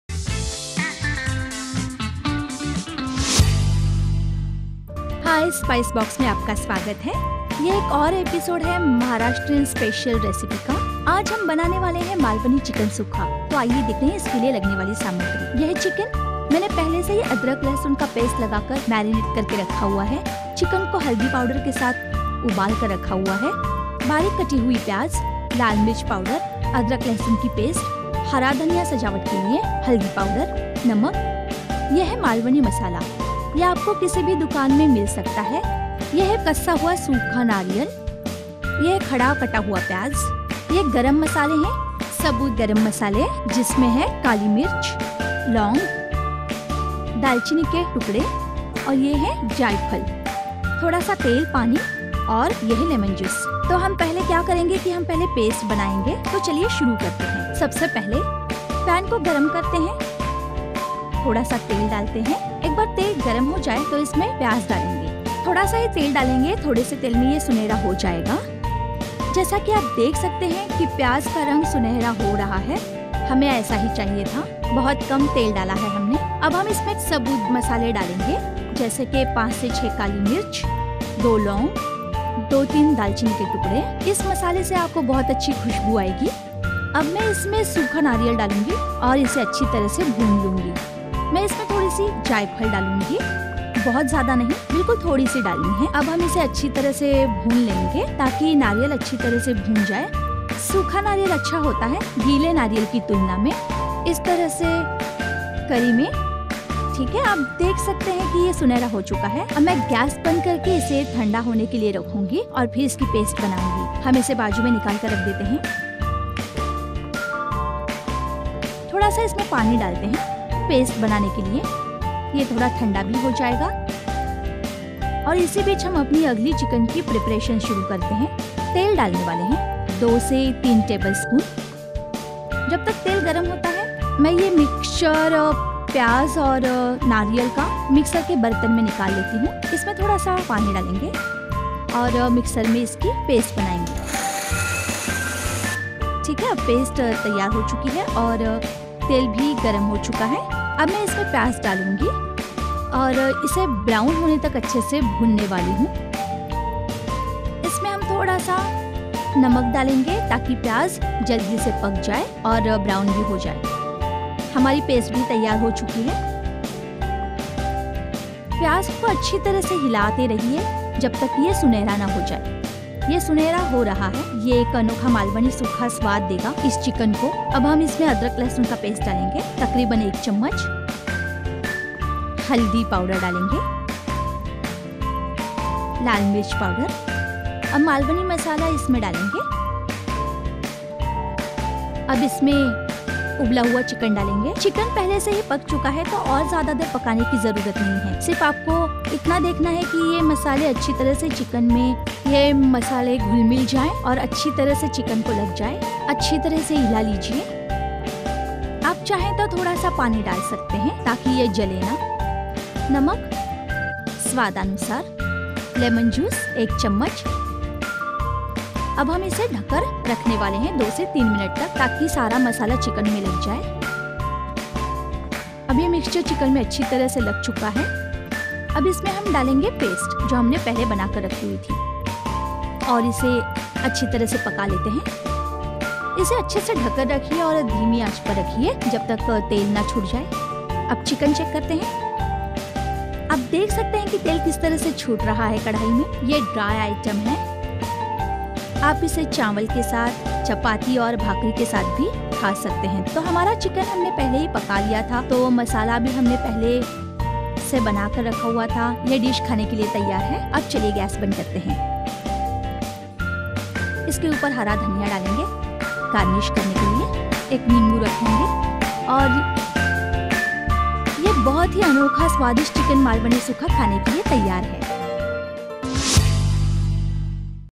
Chicken Malvani Sukha/ मालवनी चिकन सूखा
हैलो चिकन लवर्स क्या आप एक जैसा चिकन खाकर बोर हो चुके हैं और करना चाहते हैं कुछ अलग ट्राई, तो आज हम आपके लिए लेकर आए हैं मालवनी चिकन सूखा। इसे बनाना है एकदम आसान और ये खाने में है बेहद स्वादिष्ट। ये महाराष्ट्र की फेमस और ट्रेडिशनल रेसिपीज़ में से एक है।